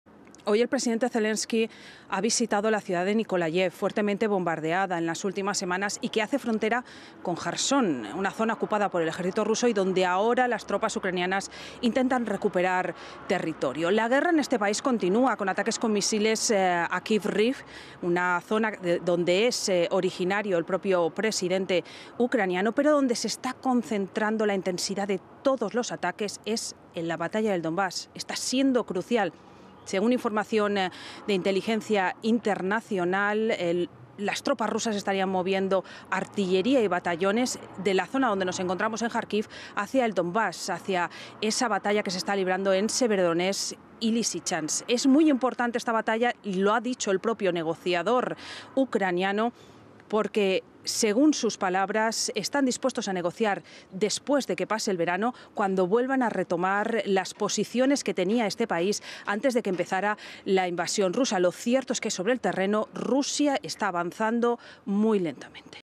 Informe desde Járkiv: Zelenski visita a los soldados en la línea del frente sur • FRANCE 24
El presidente ucraniano Volodímir Zelenski, cuyo viaje fuera de la capital, Kiev, ha sido raro desde el inicio de la invasión rusa, visitó por primera vez la localidad de Mikolaiv, en el sur de Ucrania. Un video, publicado por la Presidencia ucraniana, lo muestra inspeccionando un edificio residencial gravemente dañado y manteniendo reuniones con funcionarios locales. Mikolaiv todavía está bajo control ucraniano, pero está cerca de la región de Jerson, ocupada por los rusos. Informe de Leticia Álvarez e Irene Savio.